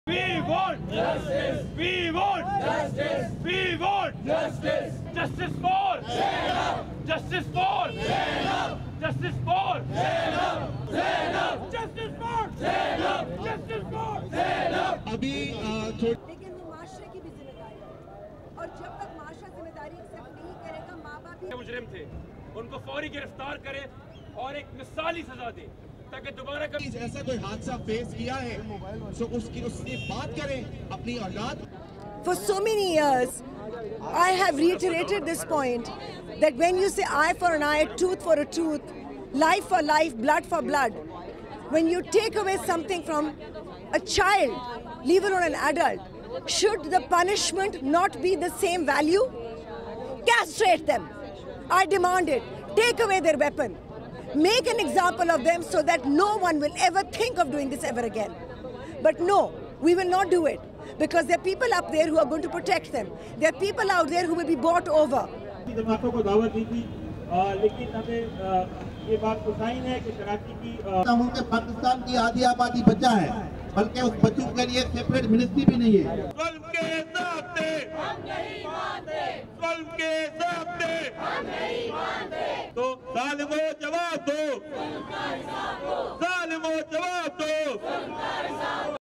Protests in Pakistan over rape and killing of 8-year-old girl
(11 Jan 2018) Protests continued on Thursday in Pakistan over the rape and killing of an 8-year-old girl in an incident that has shocked the nation.
 Zainab Ansari disappeared last week while going to a nearby home for Quranic studies and her body was found in a Kasur waste-yard on Tuesday.
Her father has accused the police of being slow to respond when his daughter went missing in eastern Punjab province.
He was on a pilgrimage in Saudi Arabia with his wife at the time of his daughter's disappearance.
Zainab's murder sparked clashes on Wednesday between angry Kasur residents and police after protesters enraged over her death attacked a police station in the city.
Two people were killed and three others were wounded in the clashes.
While on Thursday, protests occurred in both Karachi and Lahore by civil society (human rights and social) activists as they are known in Pakistan.